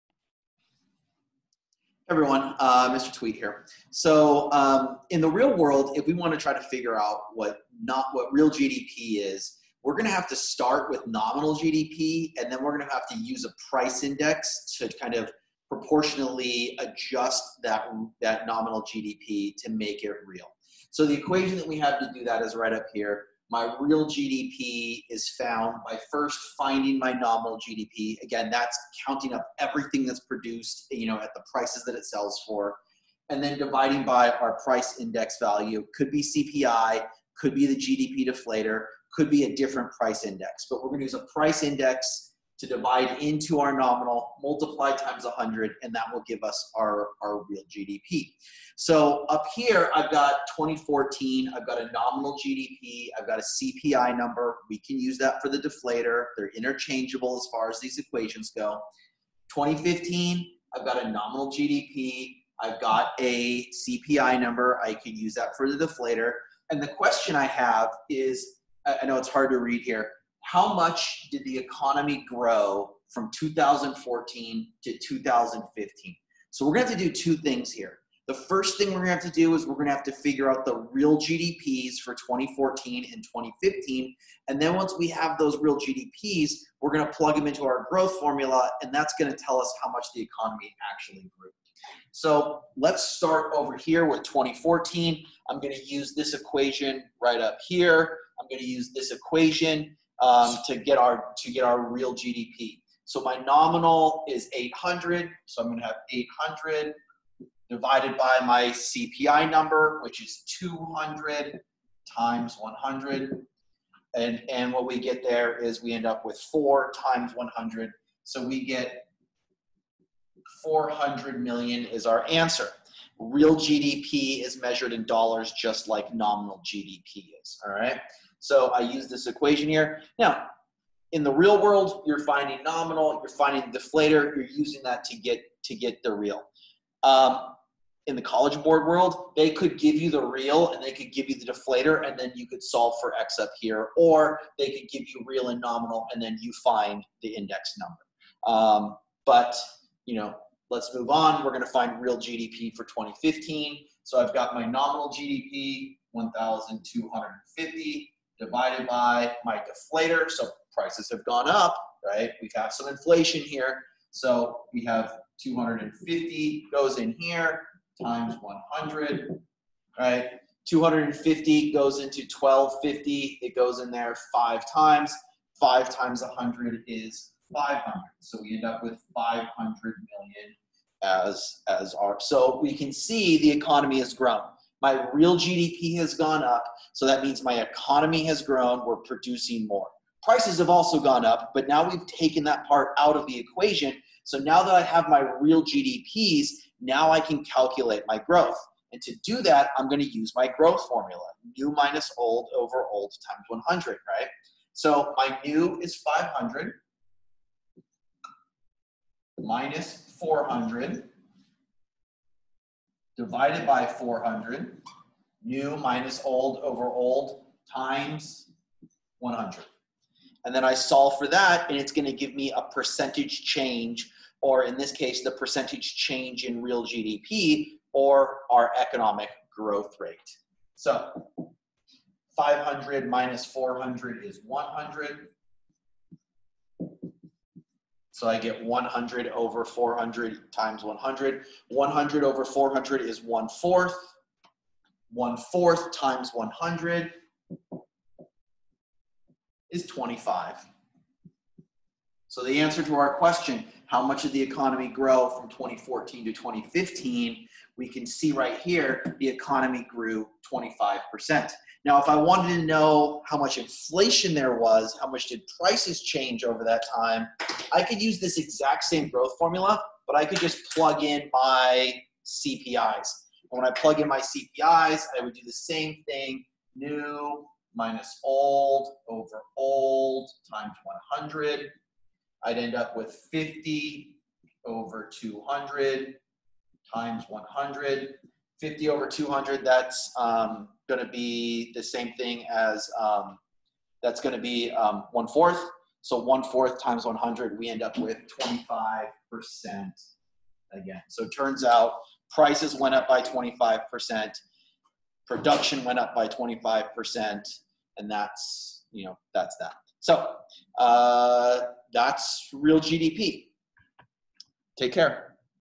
Macroeconomics: Converting Nominal GDP to Real GDP w/ price index and finding real economic growth
In this video Mr Tweet goes over the formula for converting Nominal GDP to Real GDP. Essentially, you use a price index to change the GDP measured in current, changing prices to a GDP measured in constant prices. Additionally, Mr Tweet goes over using that measure of real GDP to find real economic growth using the growth formula.

Real GDP = (Nominal GDP / GDP Deflator) * 100

Real GDP Growth Rate = 100 * (New Real GDP - Old Real GDP)/(Old Real GDP)